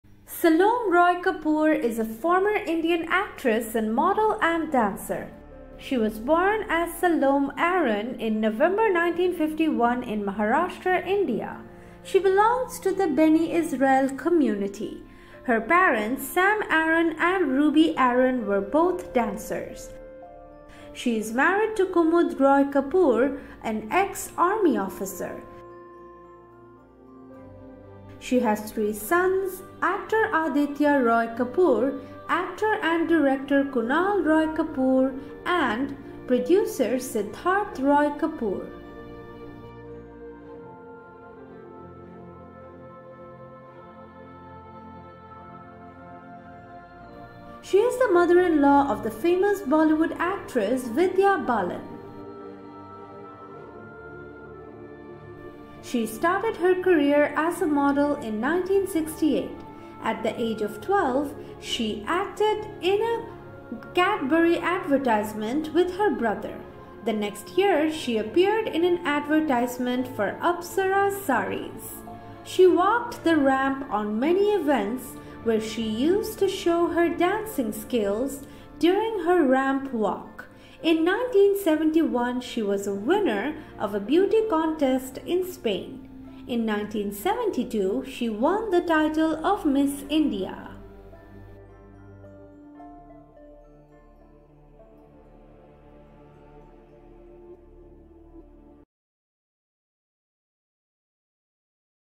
Salome Roy Kapur Family, Parents, Husband, Son
Salome Roy Kapur was born in November 1951 in maharastra, India. Her parents, Sam Aaron and Ruby Aaron were both dancers.

She is married to Kumud Roy Kapoor, a ex-Army Officer. She has three sons, Actor Aditya Roy Kapur, actor & director Kunaal Roy Kapur, and Producer Siddharth Roy Kapur.